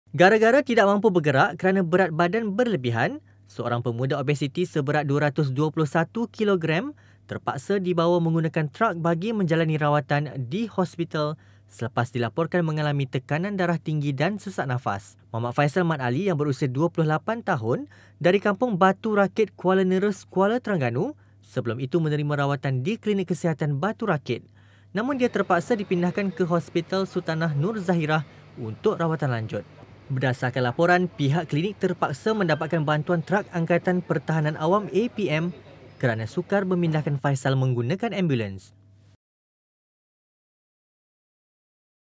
Lelaki Obes Diangkut Naik Trak Ke Hospital
PEMUDA obesiti seberat 221 kilogram terpaksa dibawa ke Hospital Sultanah Nur Zahirah (HSNZ) Kuala Terengganu menggunakan trak Angkatan Pertahanan Awam Malaysia (APM) akibat tekanan darah tinggi dan sesak nafas.

Sebelum itu Mohamad Faisal Md Ali, 28, dari Kampung Batu Rakit, Kuala Nerus, difahamkan menerima rawatan di Klinik Kesihatan Batu Rakit sebelum terpaksa dipindahkan ke HSNZ untuk rawatan lanjut.